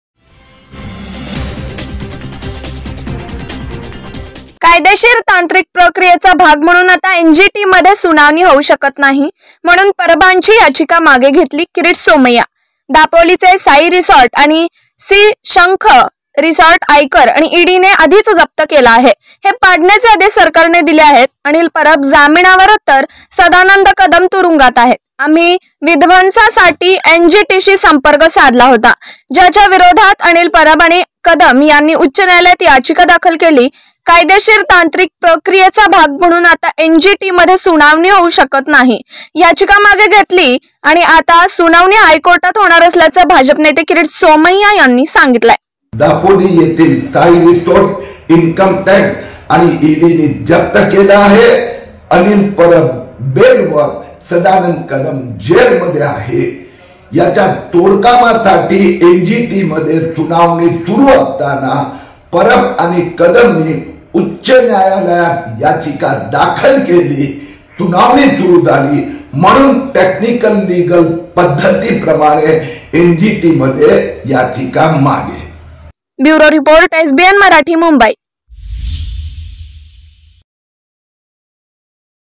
KIRIT SOMAIYA | एनजीटीमध्ये सुनावणी होऊ शकत नाही, म्हणून परबांची याचिका मागे घेतली - किरीट सोमय्या.
कायदेशीर तांत्रिक प्रक्रियेचा भाग म्हणून आता एनजीटीमध्ये सुनावणी होऊ शकत नाही, म्हणून परबांची याचिका मागे घेतली - किरीट सोमय्या.